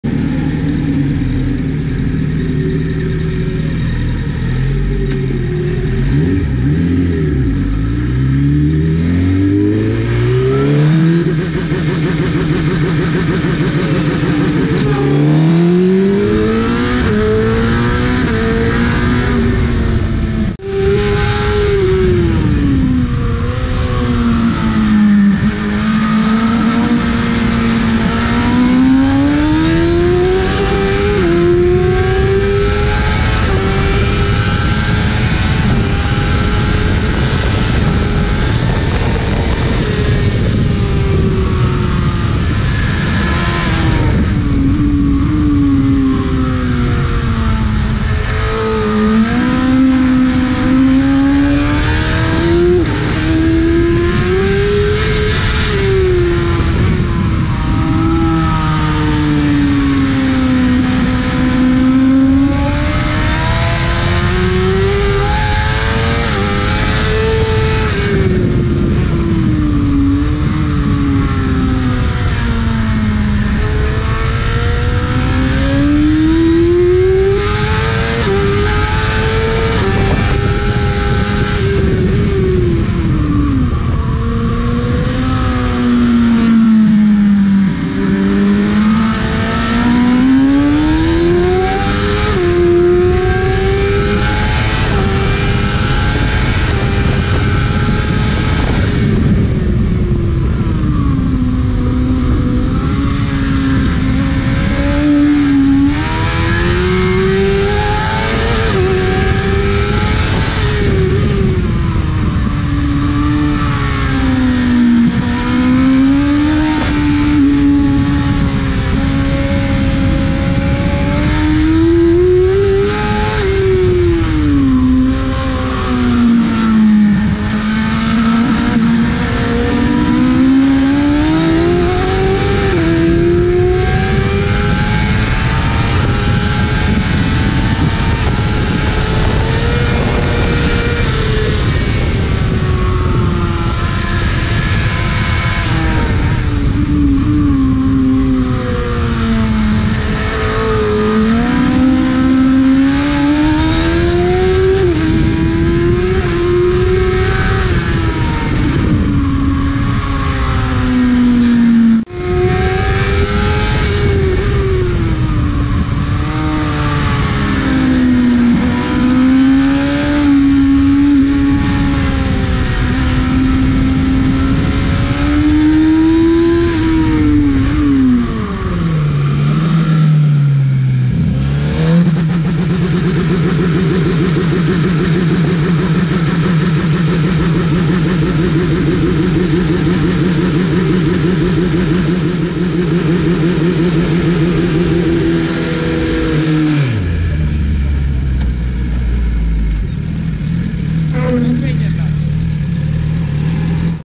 Suzuki GSXR-1000 Onboard by Night at Le Mans | Gamarino
One lap on board during the 24 Hours of Le Mans. Christian Gamarino riding the Suzuki GSX-R 1000R (World Endurance stock spec.) by team No Limits.